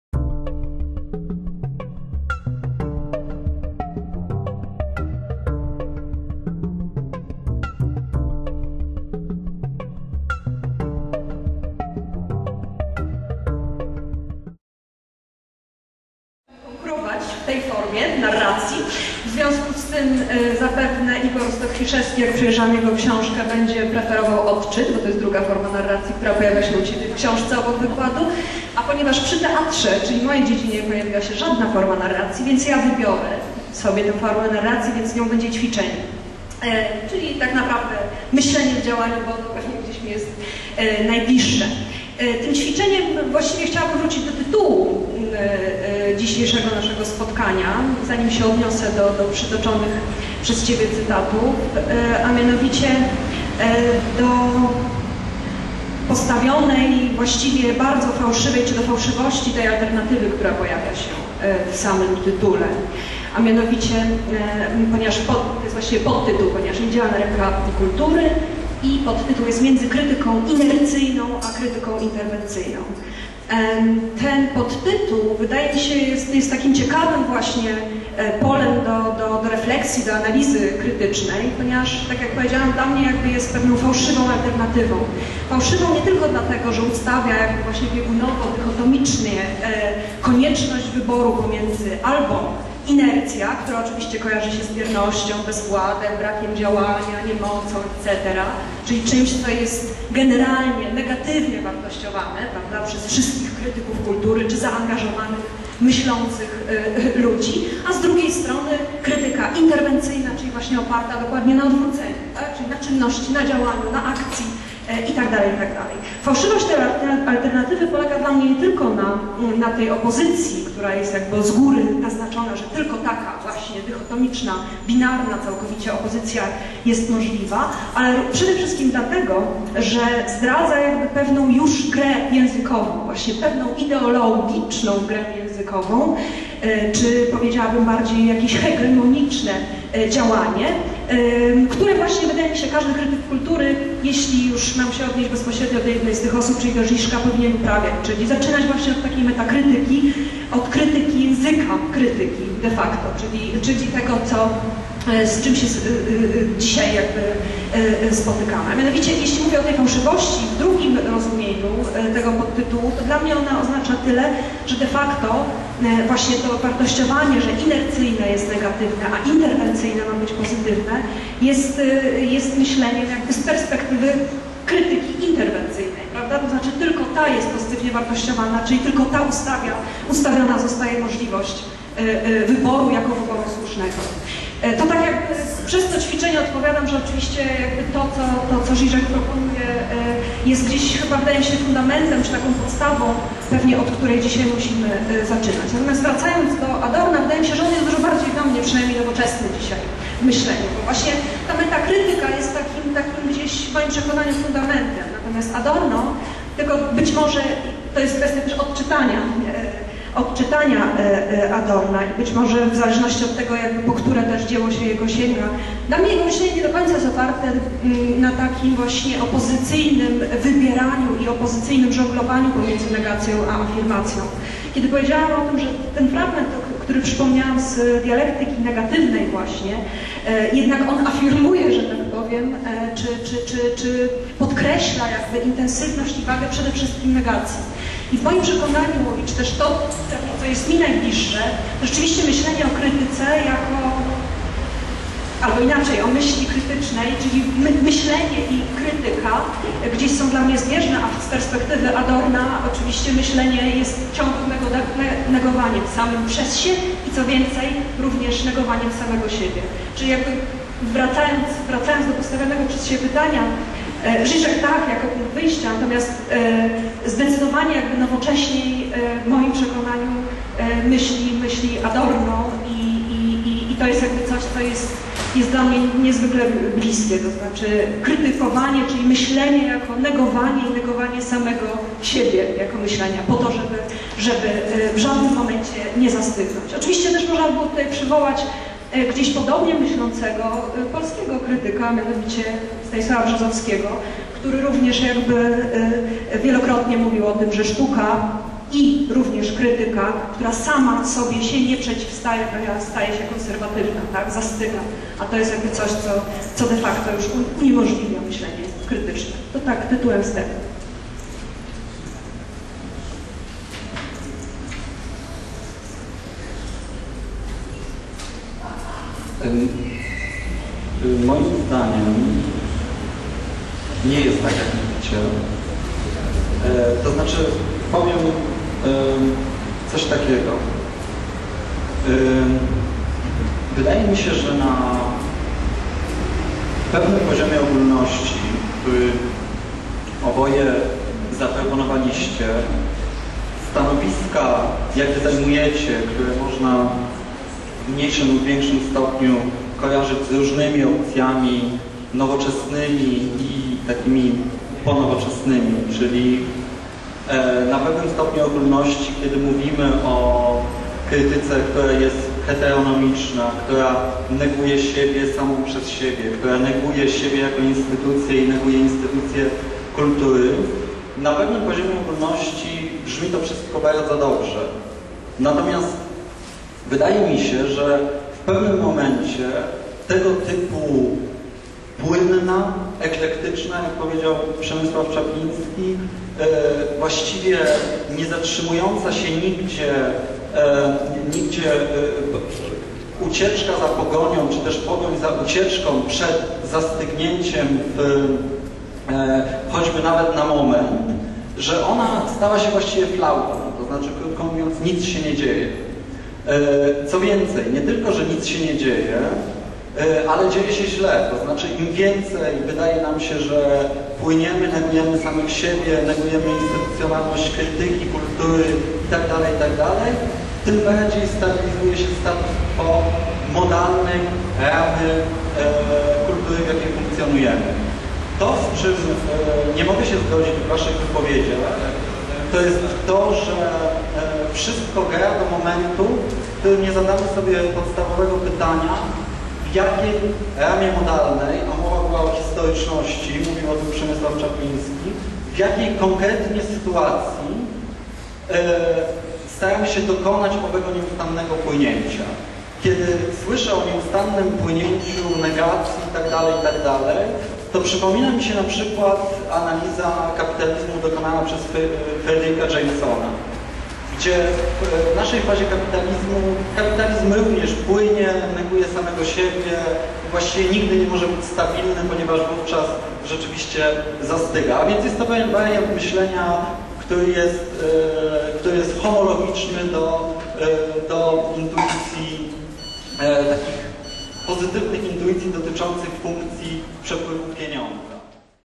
Panel dyskusyjny - Manifestacje Poetyckie 2009
Festiwal odbył się w warszawskiej kawiarni Nowy Świat. W programie m.in. prezentacje autorskie, dyskusje krytyczne, koncerty, Turniej 1 Wiersza, kiermasz książek i czasopism. W tym roku głównym punktem programu festiwalu była premiera antologii  Solistki, wydanej przez Staromiejski Dom Kultury.

Fragment panelu dyskusyjnego "Niewidzialna ręka kultury? Między krytyką inercyjną a krytyką interwencyjną.
udział biorą: Przemysław Czapliński, Dorota Sajewska, Igor Stokfiszewski.